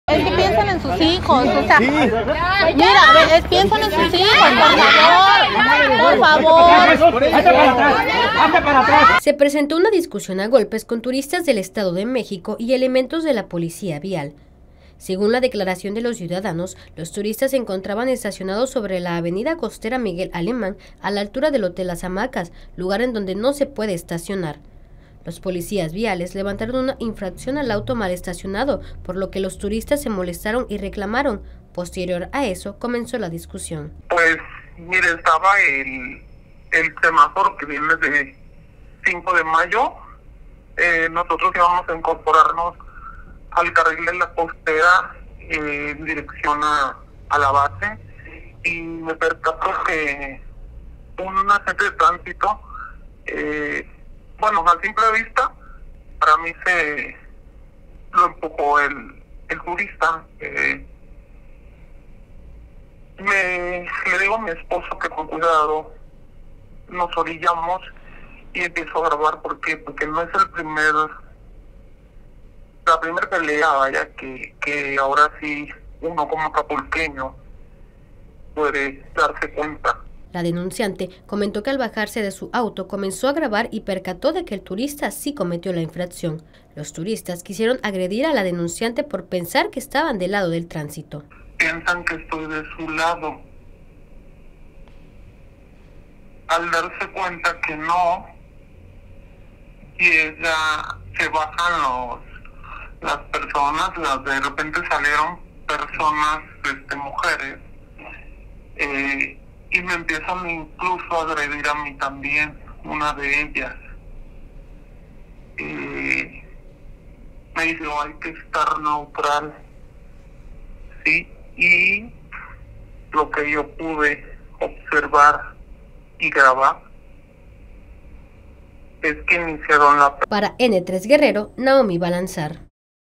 N3 Guerrero | Pleito entre turistas y policías
Elementos de la policía vial y turistas procedentes del Estado de México, se enfrascaron en una batalla campal tras una multa que fue realizada por los agentes de seguridad.

Se presentó una discusión a golpes con turistas del Estado de México y elementos de la policía vial, según la declaración de los ciudadanos los turistas se encontraban estacionados sobre la avenida costera Miguel Alemán a la altura del hotel las hamacas, lugar en donde no se puede estacionar.

Los policías viales levantaron una infracción  al auto mal estacionado por lo que los turistas se molestaron y reclamaron, posterior a eso comenzó la discusión.